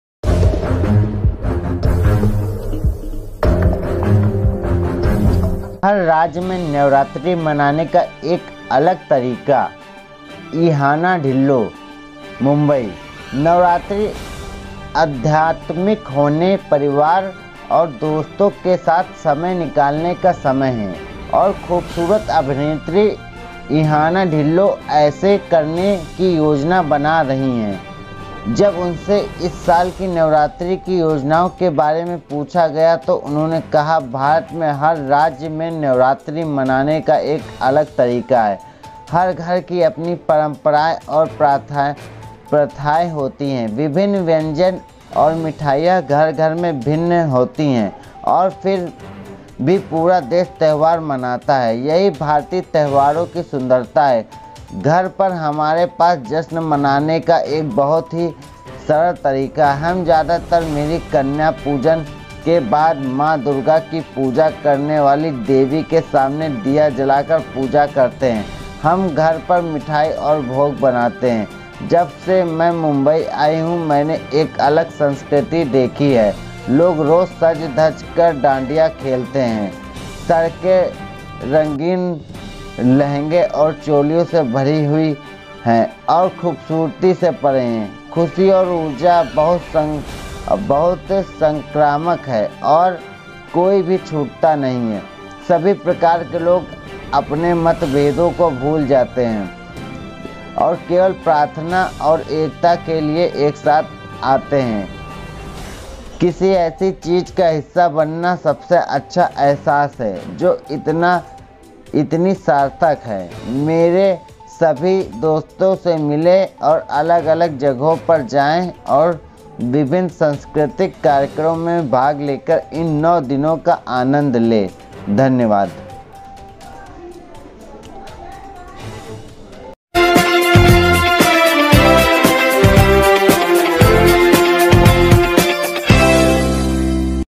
Mumbai-हर राज्य में "नवरात्रि" मनाने का एक अलग तरीका-इहाना ढिल्लों-अनिल बेदाग़-
हर राज्य में "नवरात्रि" मनाने का एक अलग तरीका-इहाना ढिल्लों

-अनिल बेदाग़-

मुंबई : नवरात्रि आध्यात्मिक होने, परिवार और दोस्तों के साथ समय निकालने का समय है। और खूबसूरत अभिनेत्री इहाना ढिल्लों ऐसा करने की योजना बना रही हैं। जब उनसे इस साल की नवरात्रि की योजनाओं के बारे में पूछा गया, तो उन्होंने कहा
"भारत के हर राज्य में "नवरात्रि" मनाने का एक अलग तरीका है। हर घर की अपनी परंपराएं और प्रथाएं होती हैं। विभिन्न व्यंजन और मिठाइयाँ घर-घर में भिन्न होती हैं और फिर भी पूरा देश त्योहार मनाता है। यही भारतीय त्योहारों की सुंदरता है। .
      घर पर हमारे पास जश्न मनाने का एक बहुत ही सरल तरीका है। हम ज्यादातर मेरी "कन्या पूजा" के बाद मां दुर्गा की पूजा करने वाली देवी के सामने दीया जलाकर पूजा करते हैं। हम घर पर मिठाई और भोग बनाते हैं।
जब से मैं मुंबई आई हूं, मैंने एक अलग संस्कृति देखी है। लोग रोज सज-धज कर "डांडिया" खेलते हैं। सड़कें रंगीन लहंगे और चोलियों से भरी हुई हैं और खूबसूरती से परे हैं। खुशी और ऊर्जा बहुत संक्रामक हैं और कोई भी छूटता नहीं है। सभी प्रकार के लोग अपने मतभेदों को भूल जाते हैं और केवल प्रार्थना और एकता के लिए एक साथ आते हैं। किसी ऐसी चीज का हिस्सा बनना सबसे अच्छा अहसास है जो इतनी सार्थक है। मेरे सभी दोस्तों से मिलें और अलग-अलग जगहों पर जाएं और विभिन्न सांस्कृतिक कार्यक्रमों में भाग लेकर इन 9 दिनों का आनंद लें।"